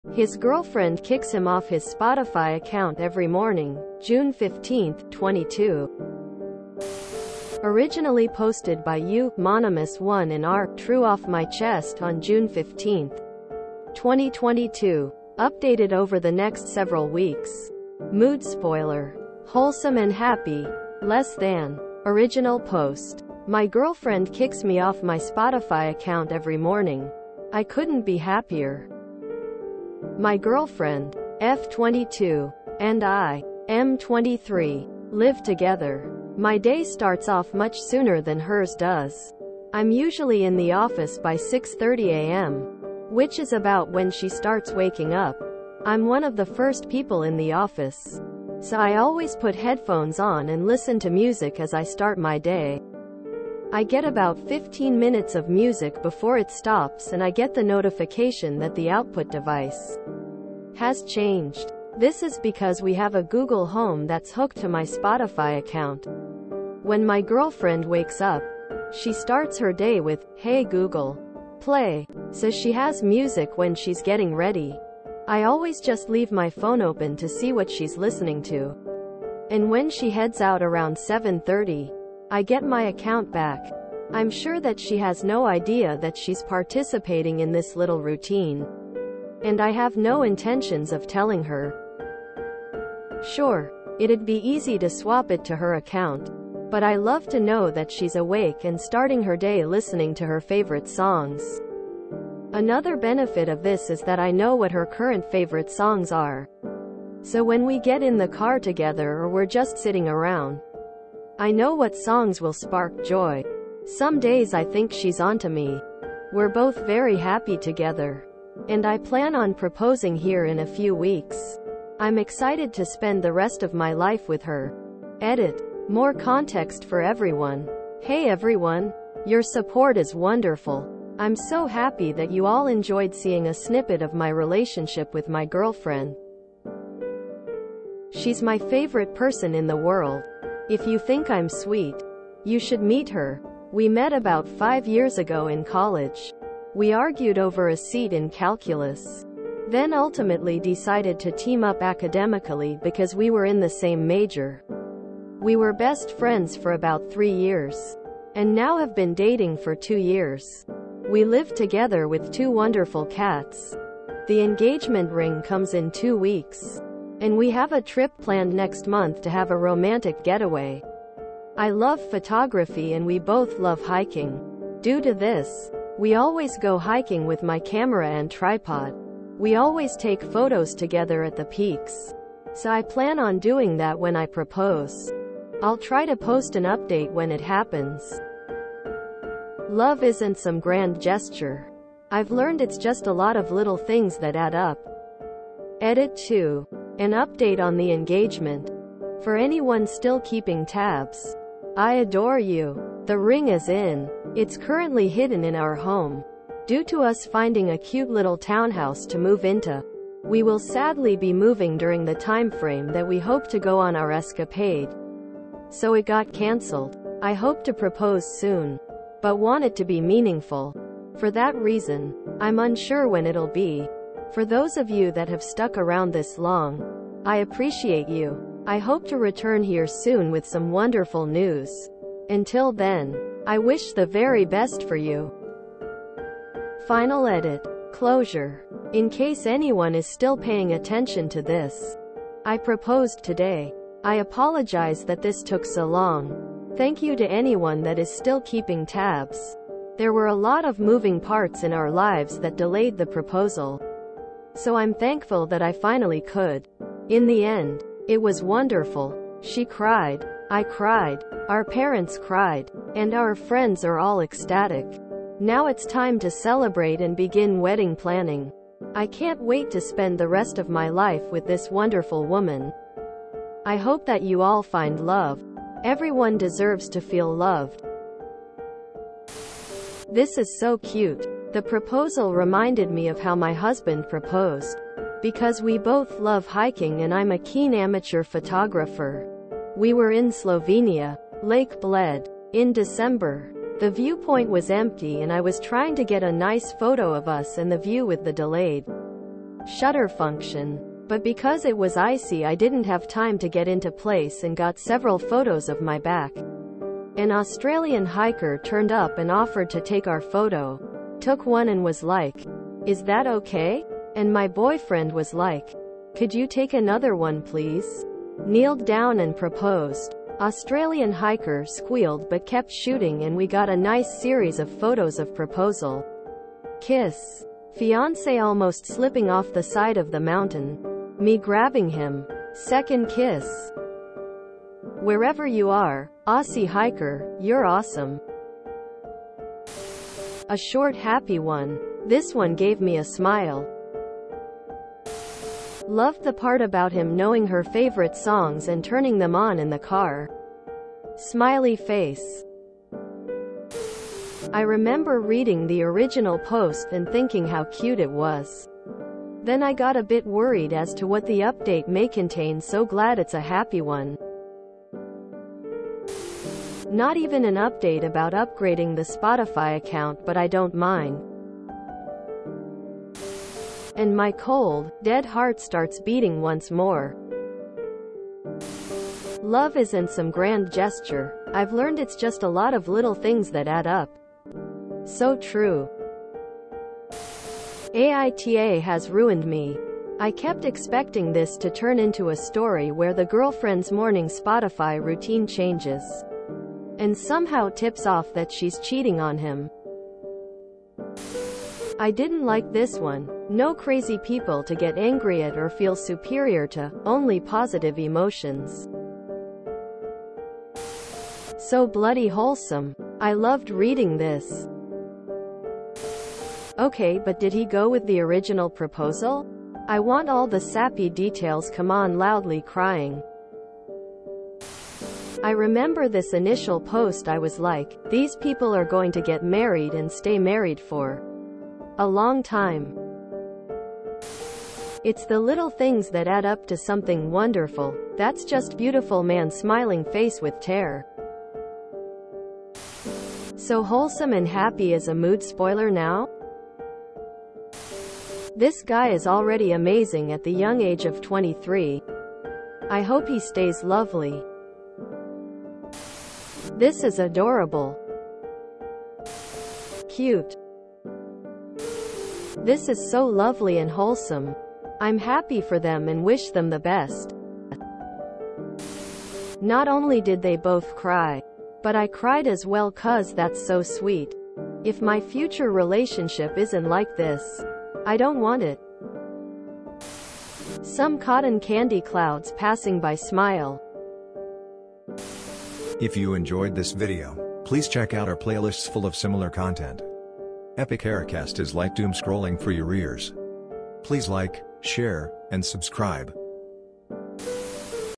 His Girlfriend Kicks Him Off His Spotify Account Every Morning (June 15, '22)
His Girlfriend Kicks Him Off His Spotify Account Every Morning (June 15, '22) (r/BestofRedditorUpdates)

Source: KittenDealinMama on Reddit (link removed at the insistence of YouTube)